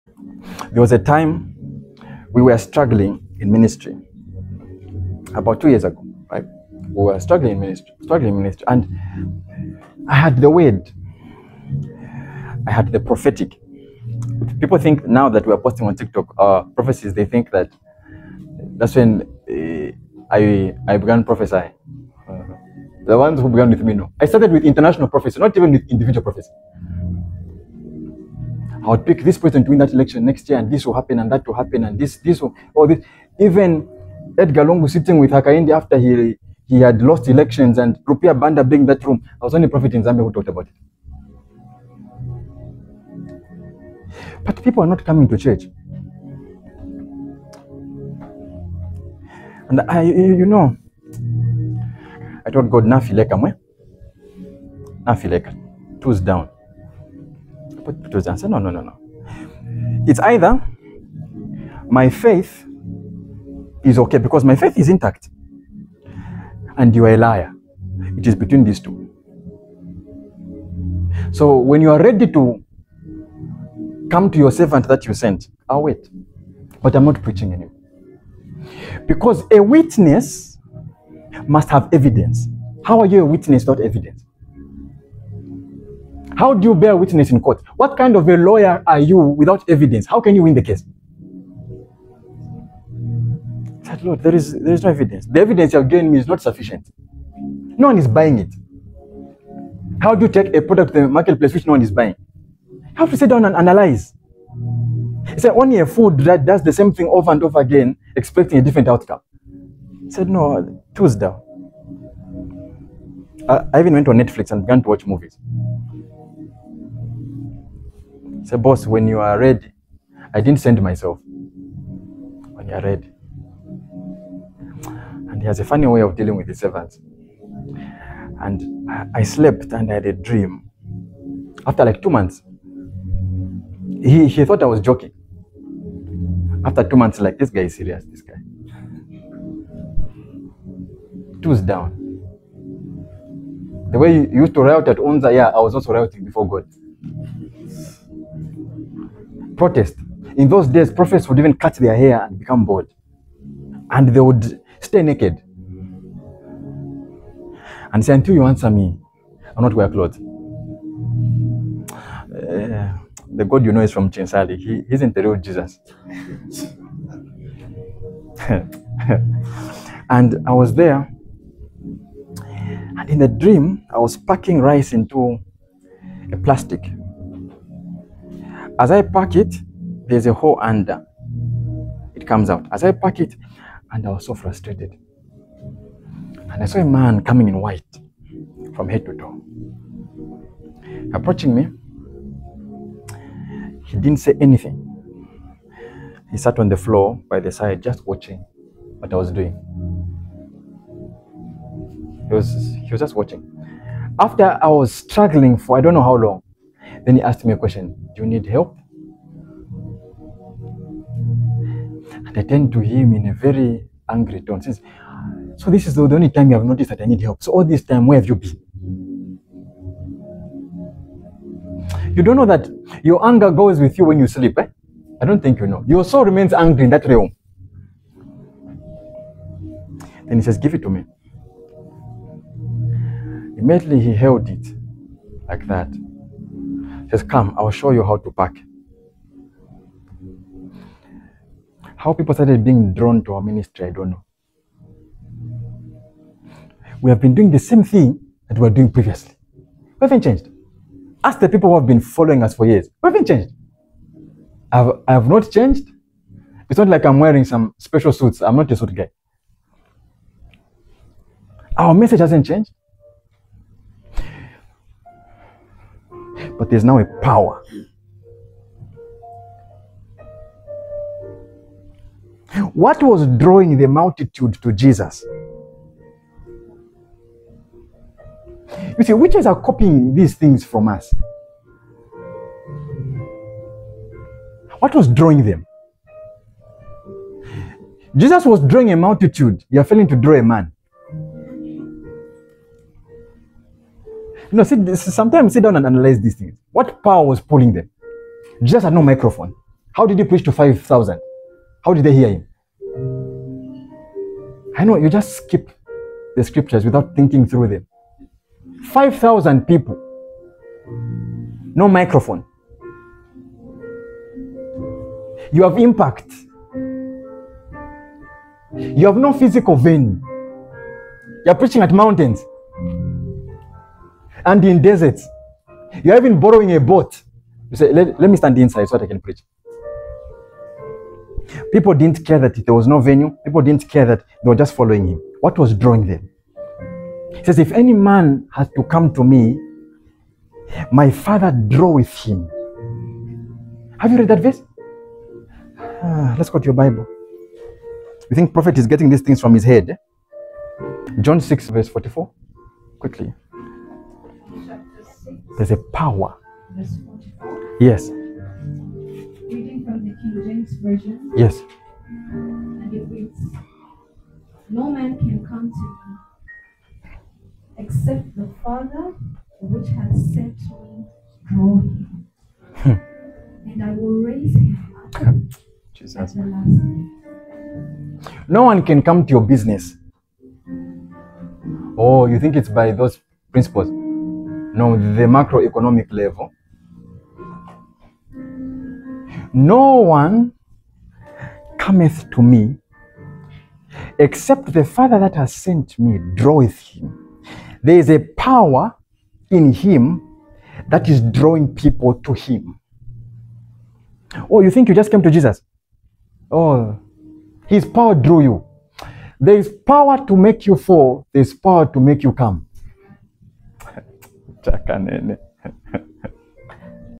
WHEN I QUIT MINISTRY 🥹
Our Man of God, Prophet Israel Lubuto, describes his Journey in Minisyry, and What made him Quit, and what happened afterwards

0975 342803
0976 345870